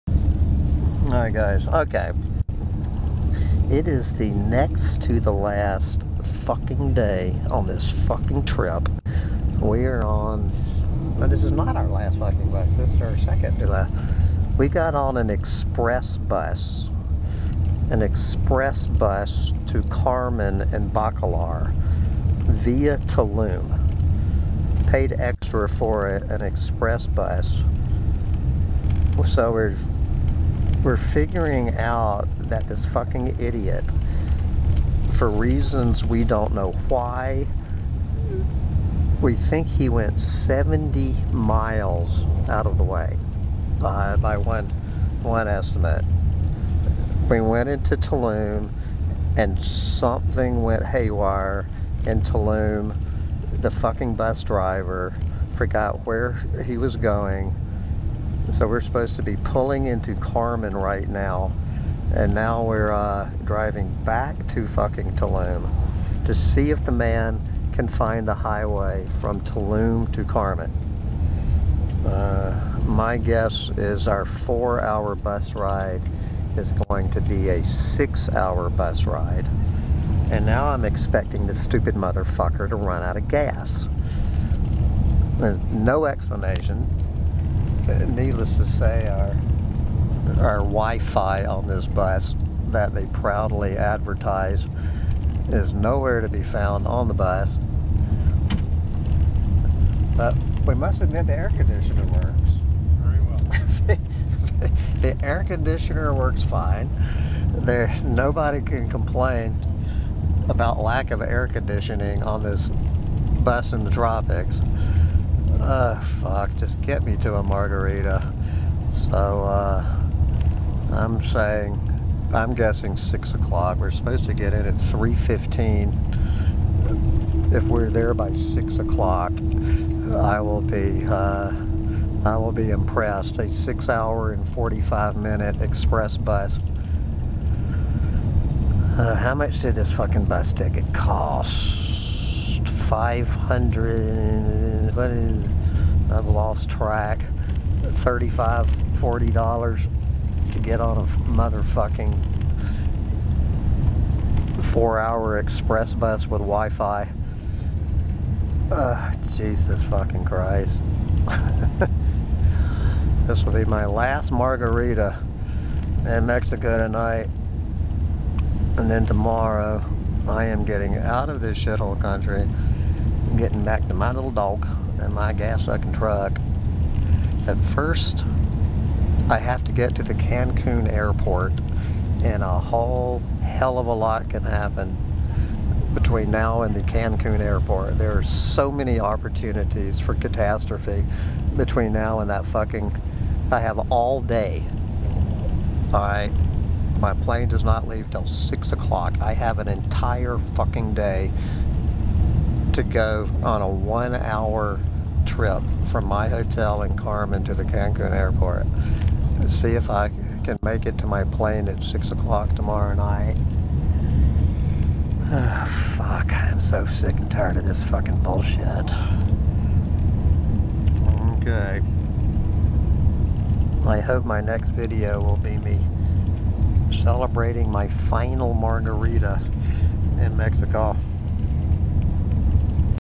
Back in Mexico, Ch. 32: Depressed Doomsday Tourist Hops on an Express Bus to Hell With Moron Driver
I hope this clueless moron motherfucker is not going to be the pilot on my plane to Austin, or we'll end up in LA.